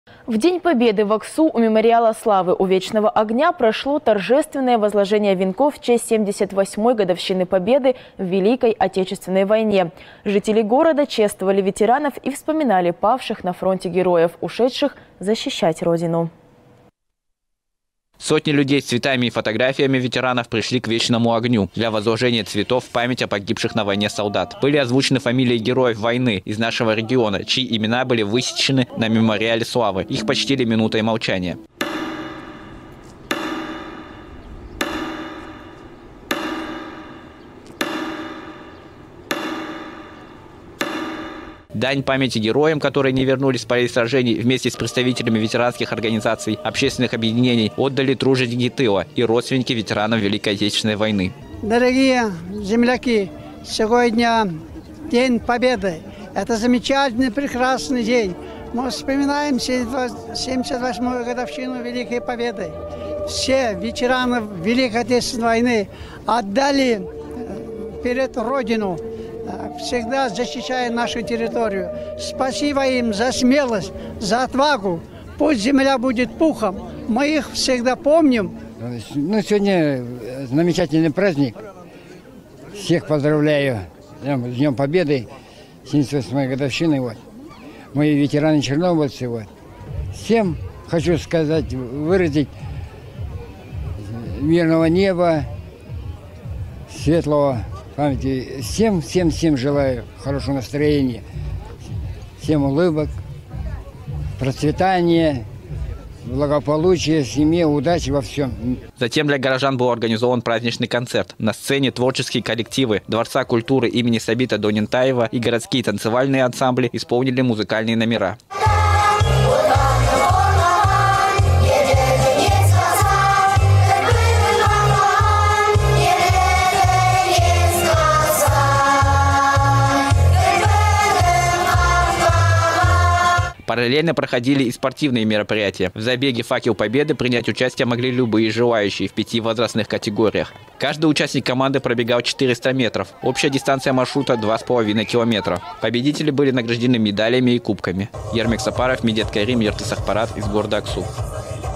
Дань уважения ветеранам
В День Победы в Аксу у мемориала Славы у Вечного огня прошло торжественное возложение венков в честь 78-ой годовщины Победы в Великой Отечественной войне. Жители города чествовали ветеранов и вспоминали павших на фронте героев ушедших защищать родину.